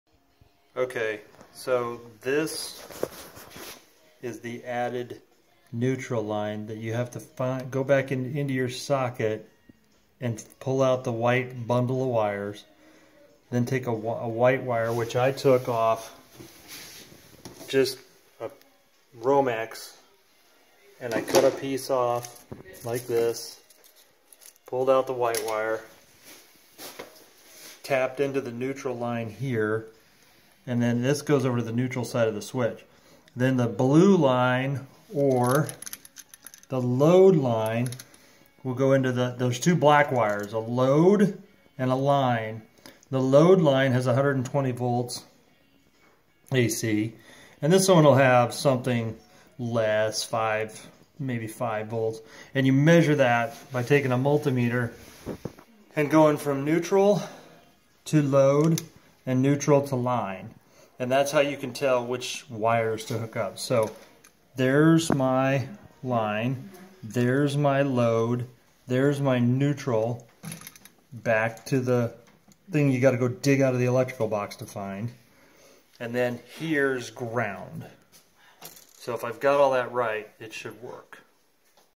Installing a programmable switch
My porch light after shutting it off would constantly flicker. Adding a neutral line to the existing circuit enables the switch to work without flickering.